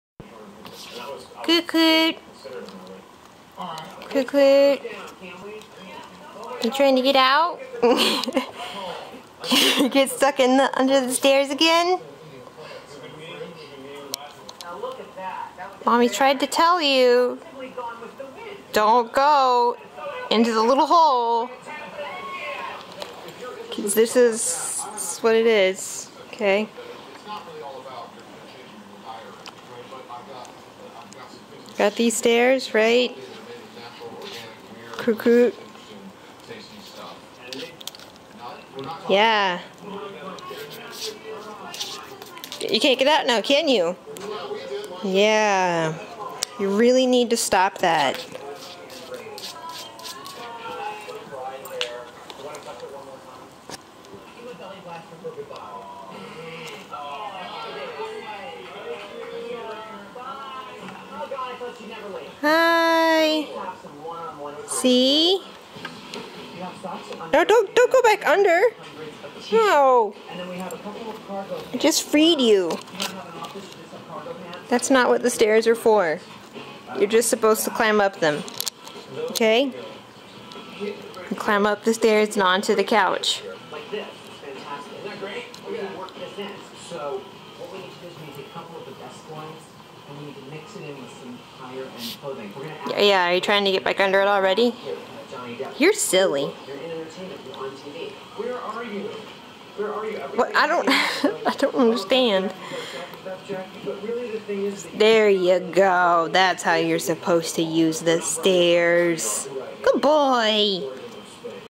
Trapped In the Stairs
My ferrets keep getting under their little steps. It's like they know they are going to get stuck, but the do it anyways!